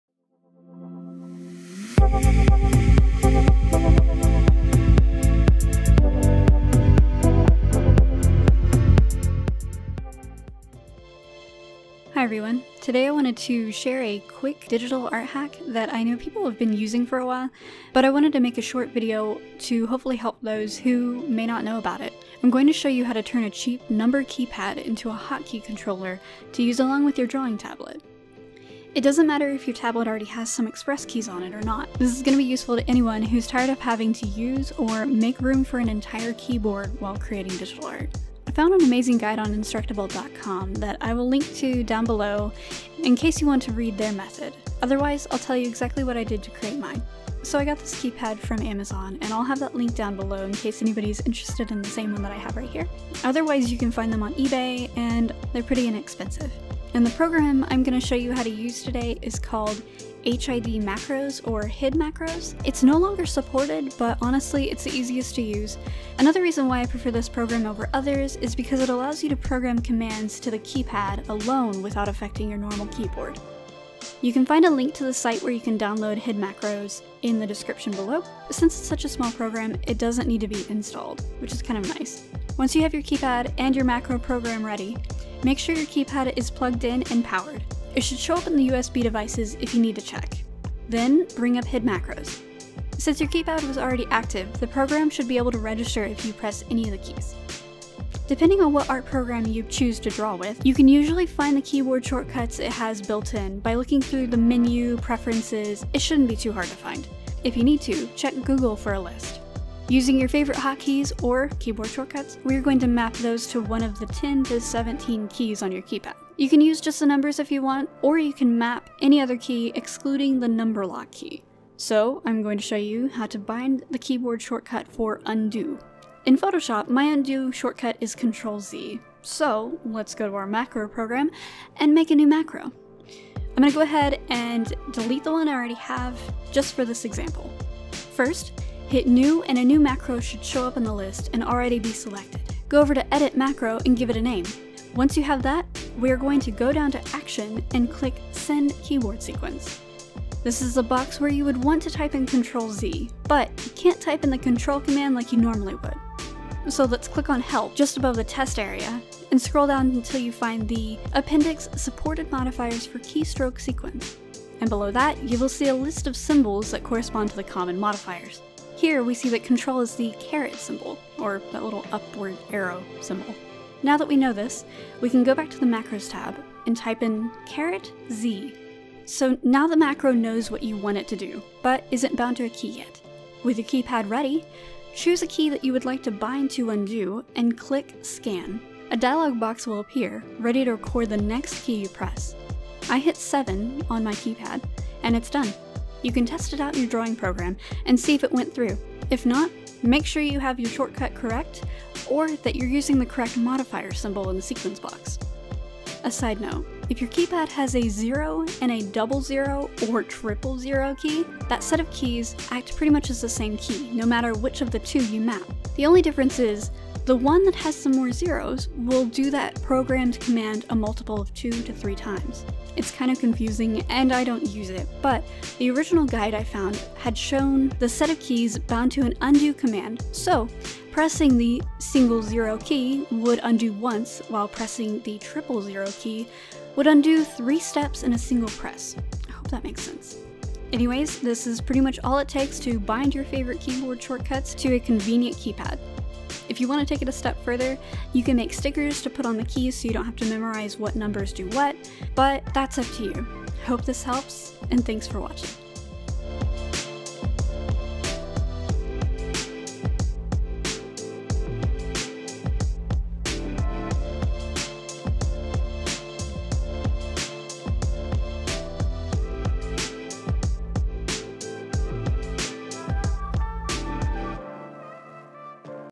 DIY Custom Keypad for Digital Art
Hi everyone!
I have a couple of budget tablet reviews coming up very soon and I thought I'd go ahead and show you how to make your own keypad 'expresskey remote' in case your tablet lacks enough/any buttons to make you digital art life easier!

[Tip: be sure to click "version 2.3" on the download page in order to get HID instead of LUA]

Mac Users! A similar program to HID Macros [though I've not tested it myself]

Common question: How do I bind the space bar? You can't, instead, change the shortcut from 'space bar' in your program you are using to another key [such as 'a'] and bind that to the keypad instead.

Too complicated or need way more keys than a numpad?
Try one of these instead

▶ Art Supplies
▶ Filming Equipment